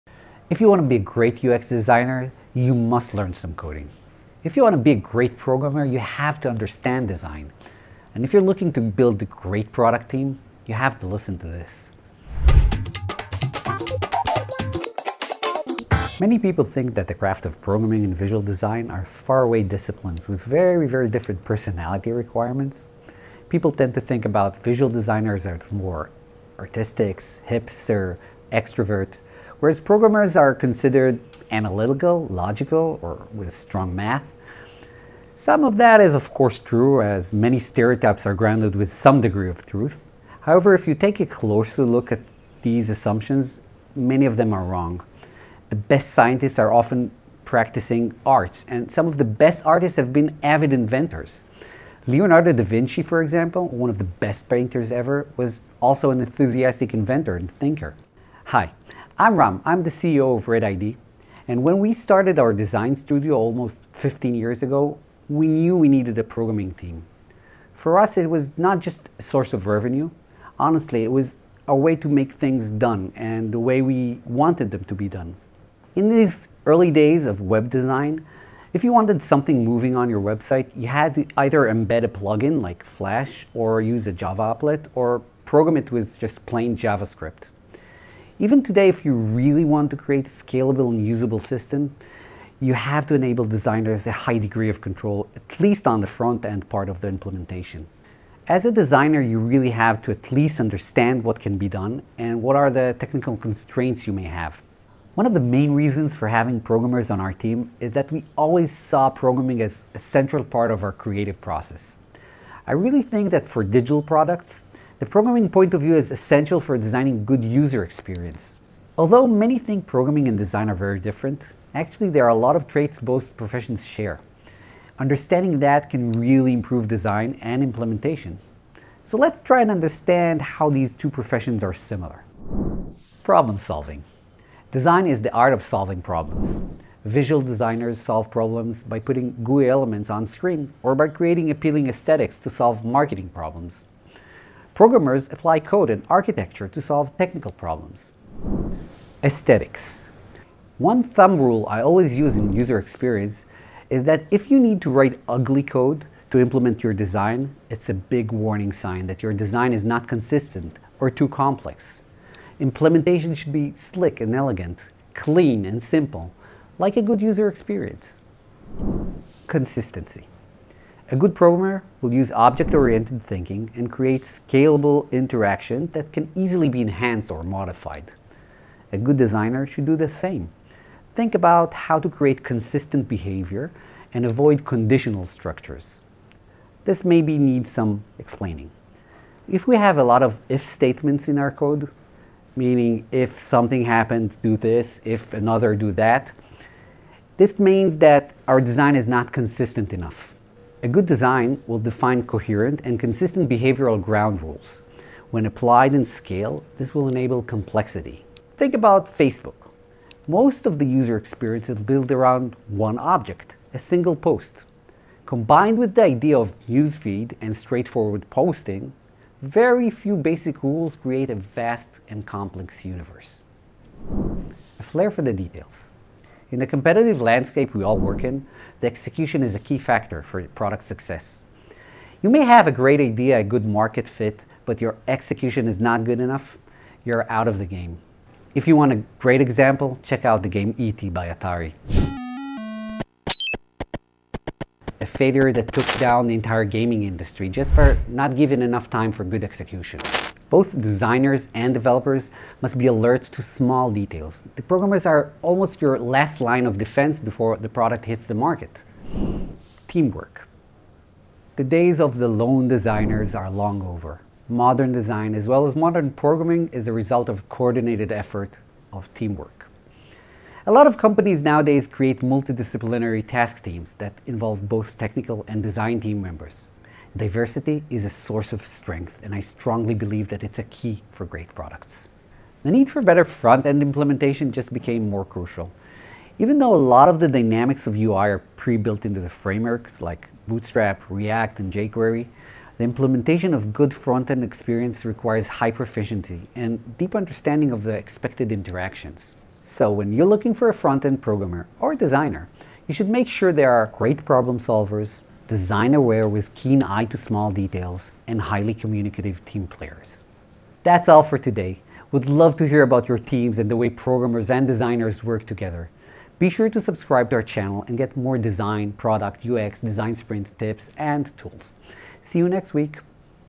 Why should designers know code and programmers know design? Design-Aware Programming!
If you want to be a great UX designer, you must learn some coding. If you want to be a great programmer, you have to understand design and if you are looking to build a great product team - you have to listen to this!